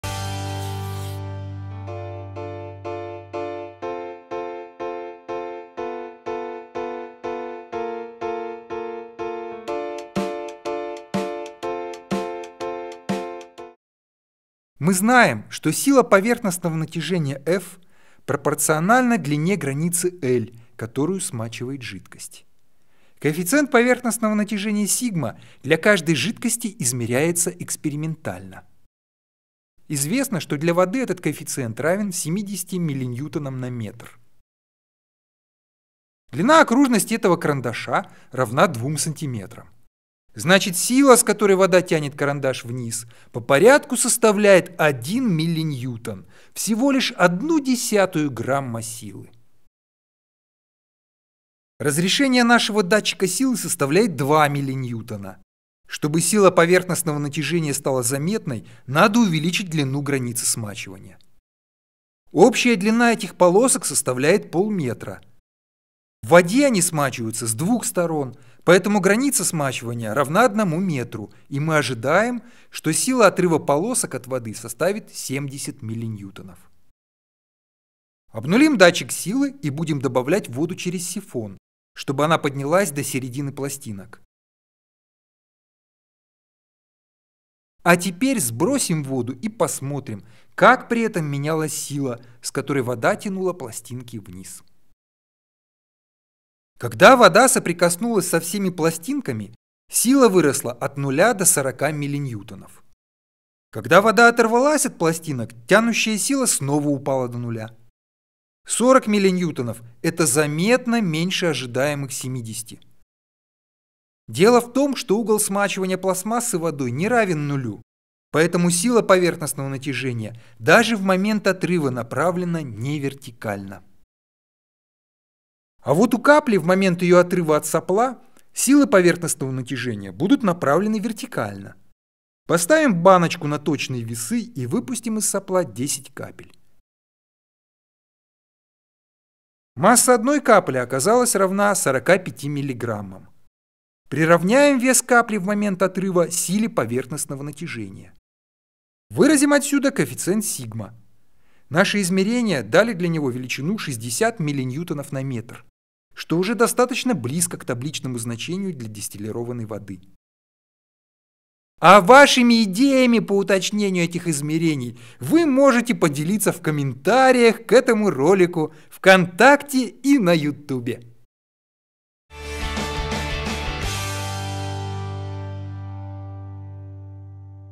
Коэффициент поверхностного натяжения
Рассматриваются два метода измерения коэффициента поверхностного натяжения: по отрыву и по счёту капель.